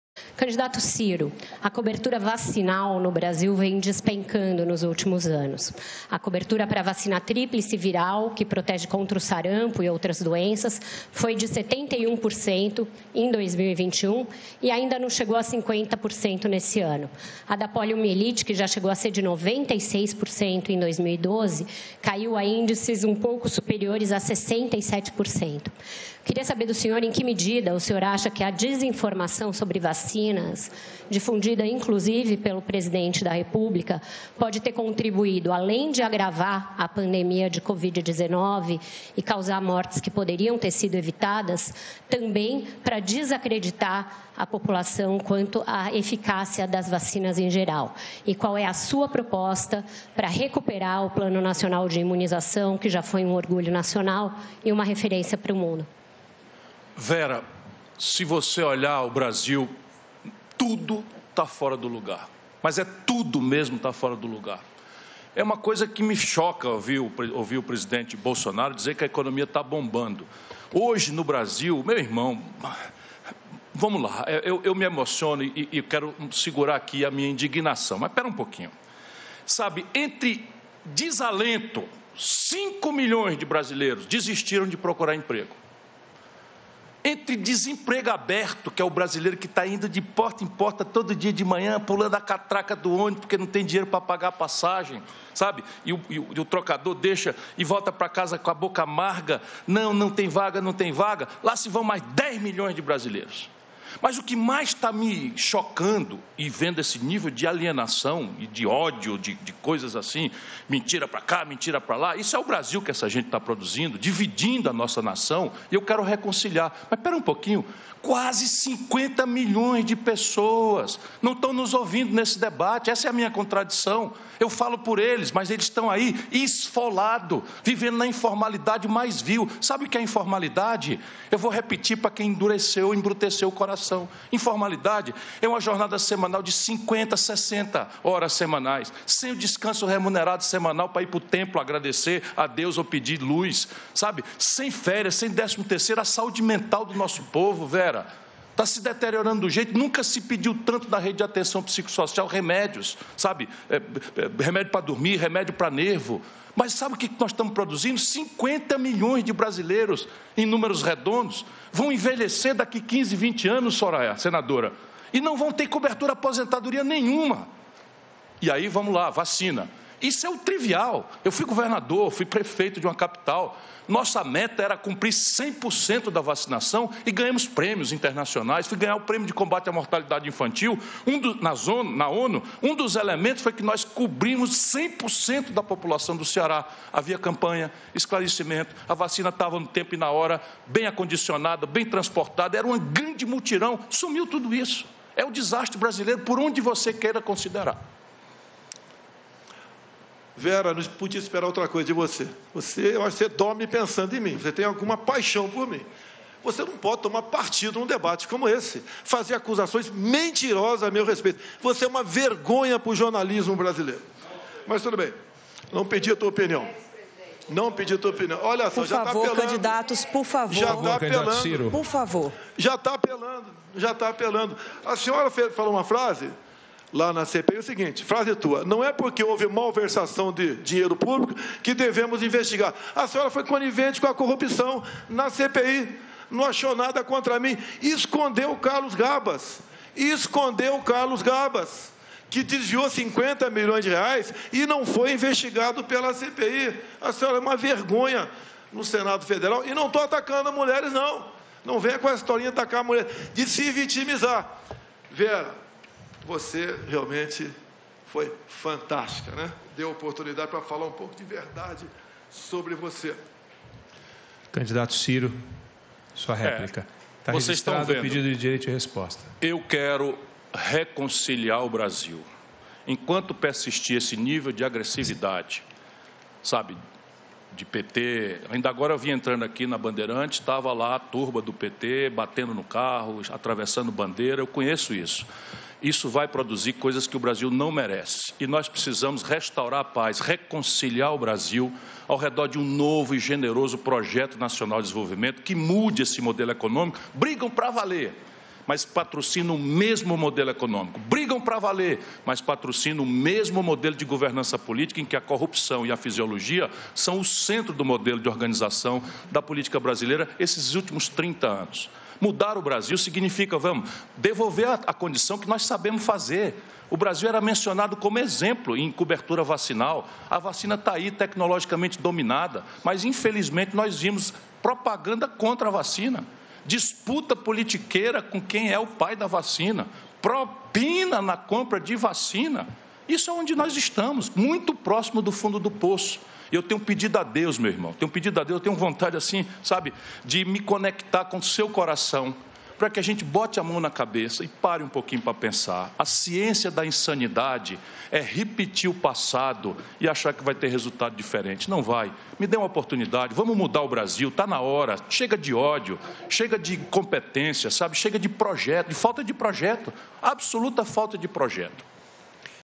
Vera Magalhães questiona Ciro Gomes sobre queda vacinal no Brasil
A jornalista Vera Magalhães perguntou ao candidato à Presidência Ciro Gomes sobre a situação da cobertura vacinal no Brasil. O presidente Bolsonaro foi escolhido para comentar a resposta.

Confira as declarações.

Acompanhe o debate ao vivo pelas redes da TV Cultura.